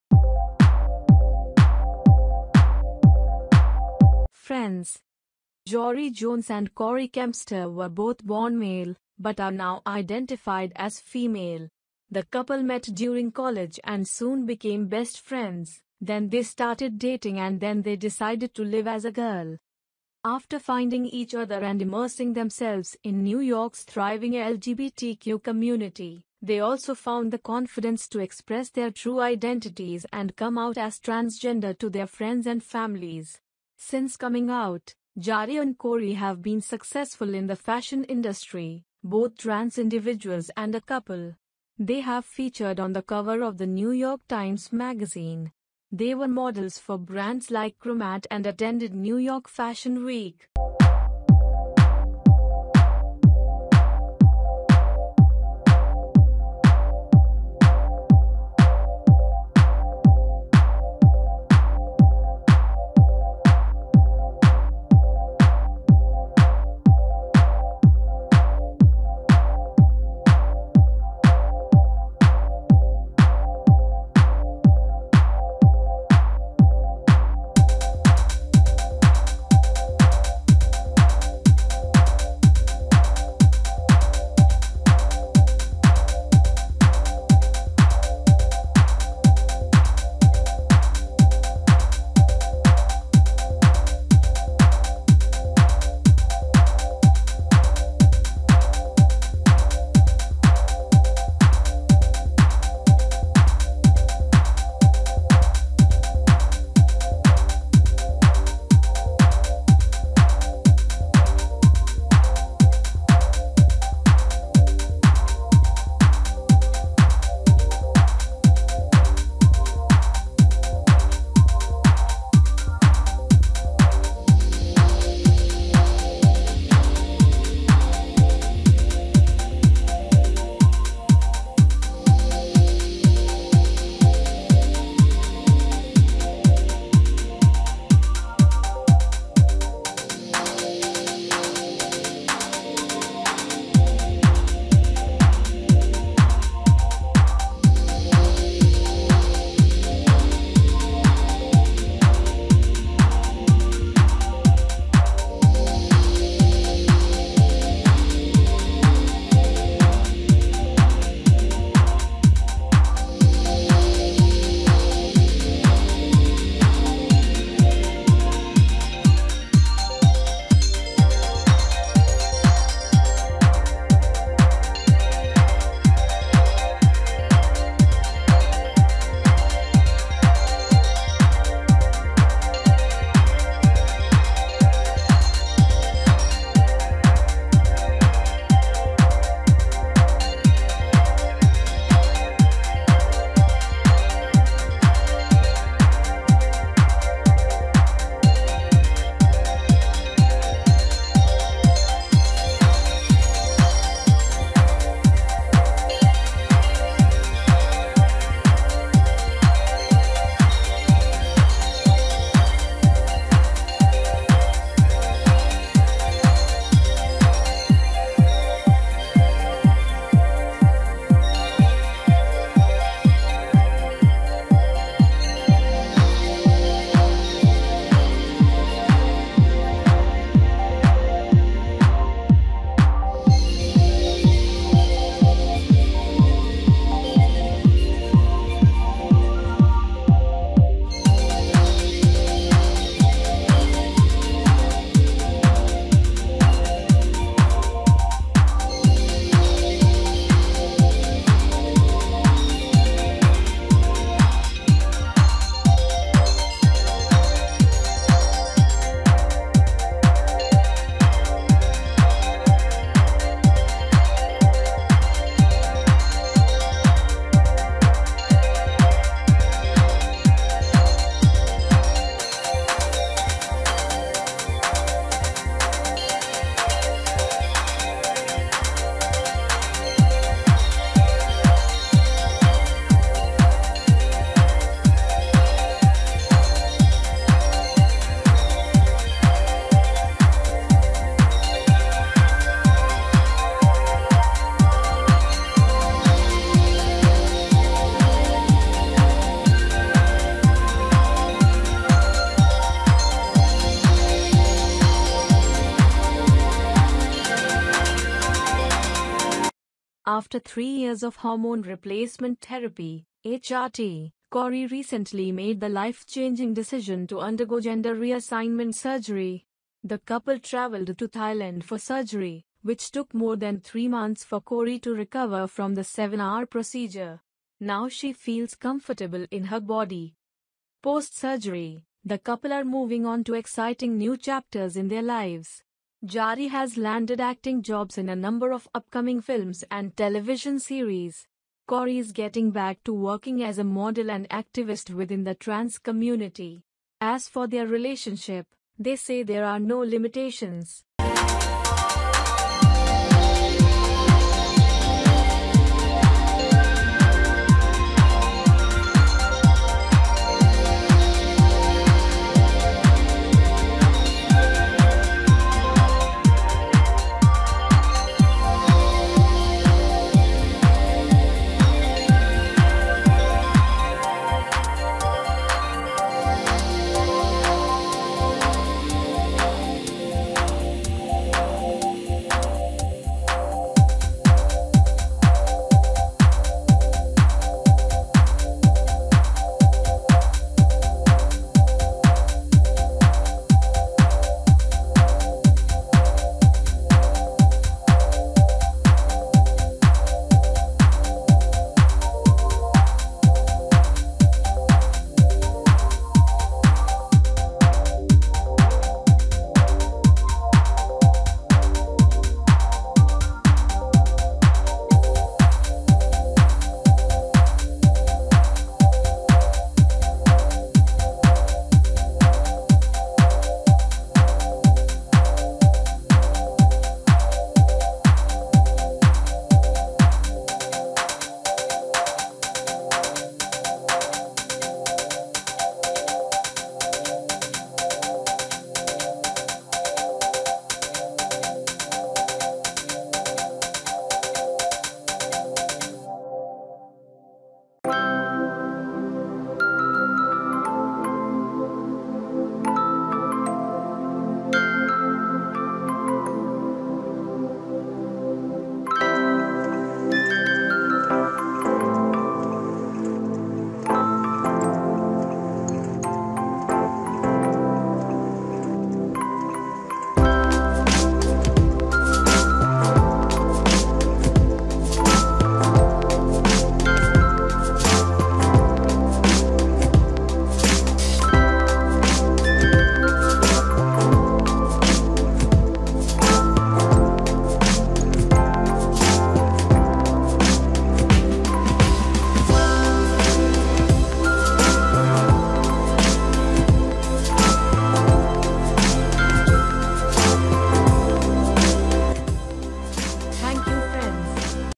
Jari and Corey relationships started as boy friends and now they have become girlfriends
Jory Jones and Corey Kempster were born male.The couple met during college and soon became best friends. They started dating.They started to identify themselves as women and become women.Jari and Corey have made their mark in the fashion industry.They have featured on the cover of New York Times Magazine.